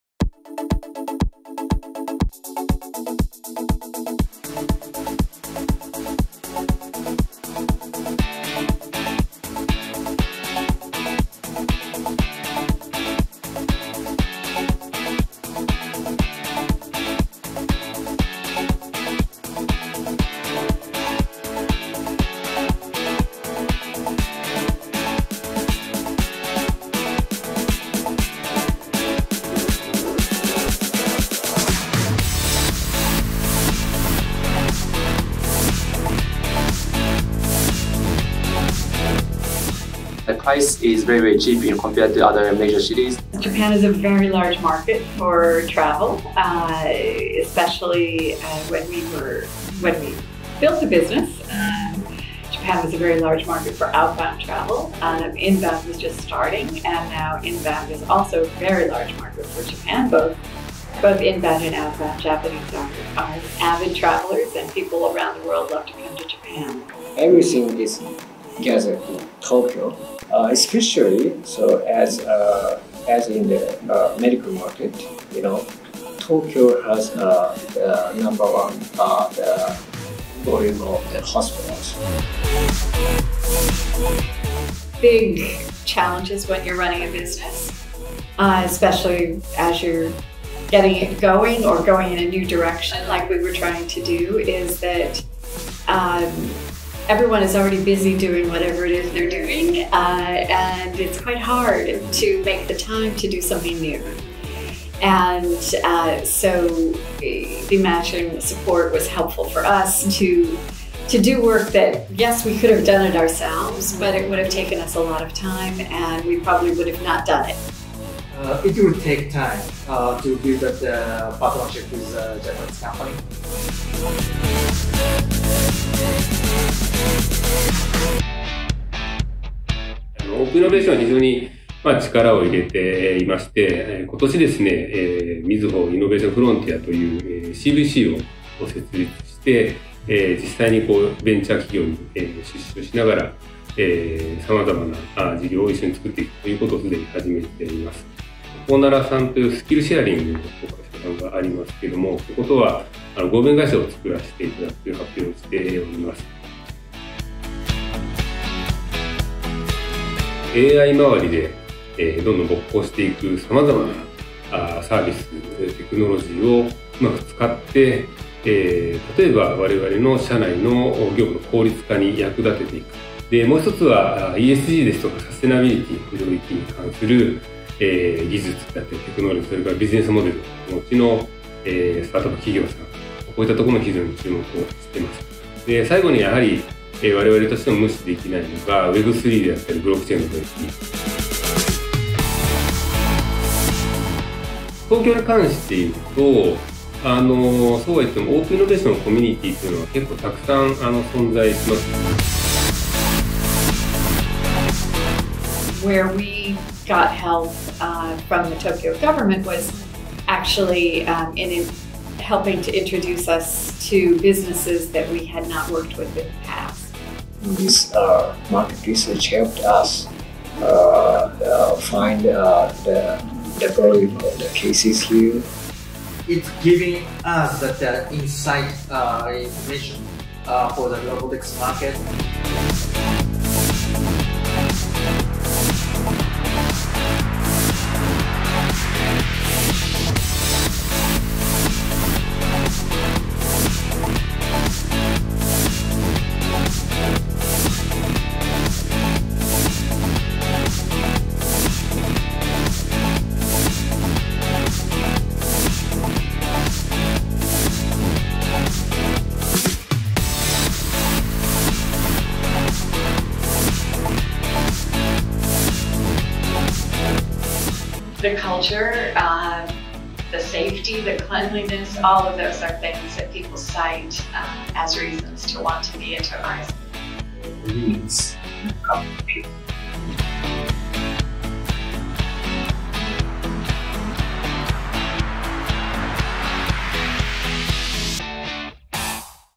Invest Tokyo：TRUE STORY of Tokyo Long ver.
TRUE STORY of TOKYO talks about the true attractiveness of Tokyo.
In this video, we welcomed global companies that already expanded into Tokyo with the Tokyo Metropolitan Government support for their business expansion and Japan's major financial institution. You can learn about essential information for your business expansion!